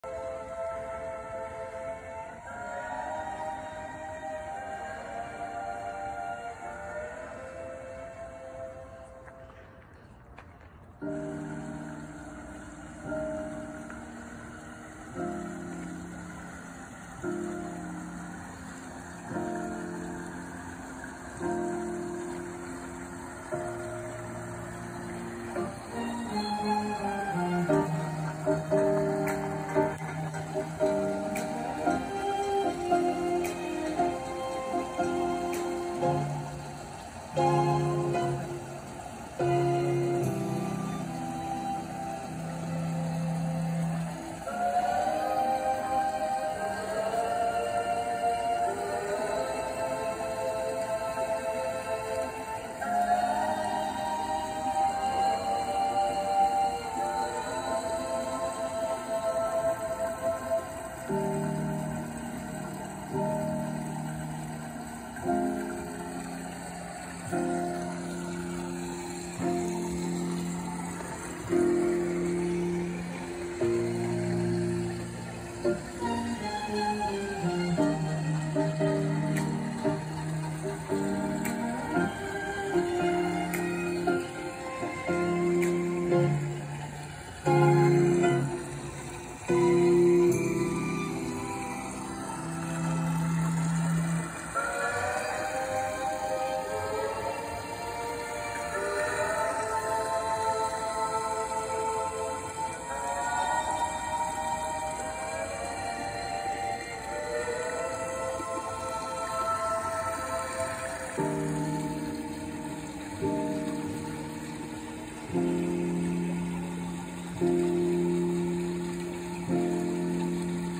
Naked夜祭り高知城No. 5
高知城で行われている光のイベント。
This is the lighting event at Kochi Castle.